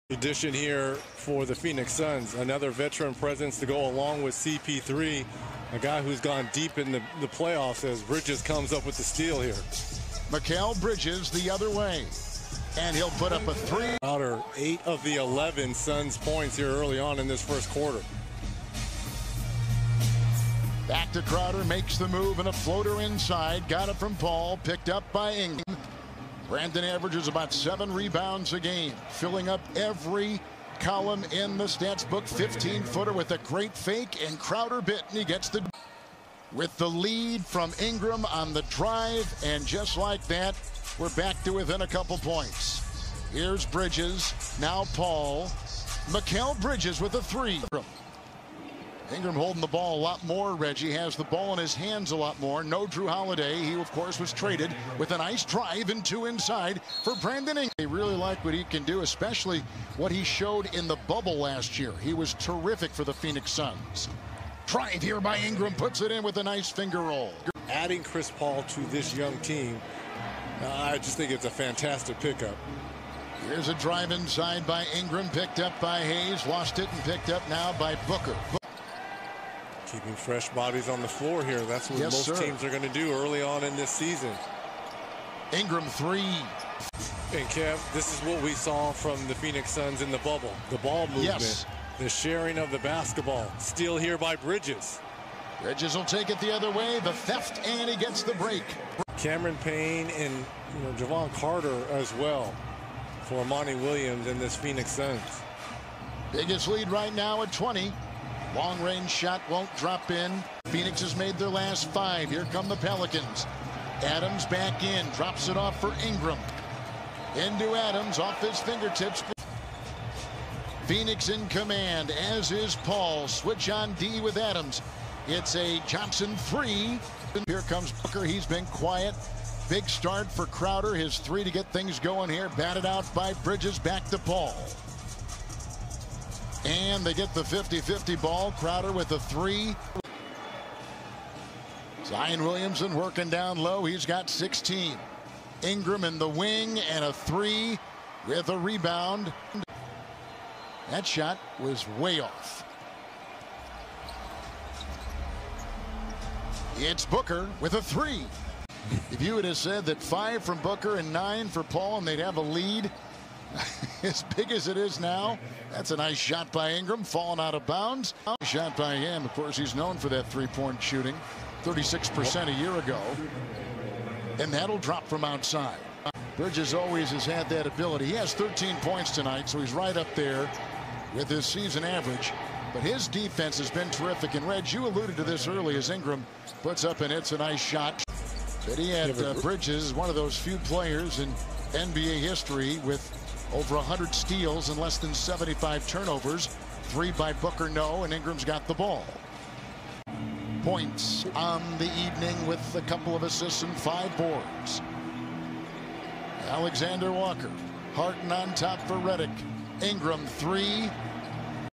Brandon Ingram 13 PTS 6 REB 6 AST: All Possessions (2020-12-30)
Like and sub for more highlight videos. Comment if you want a specific player.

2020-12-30 Phoenix Suns vs New Orleans Pelicans: Player Highlights Brandon Ingram
    13 PTS, 6 REB, 6 AST, 1 STL.
        1 of 5 from 3.
        6 of 11 FGs in 28:14 minutes.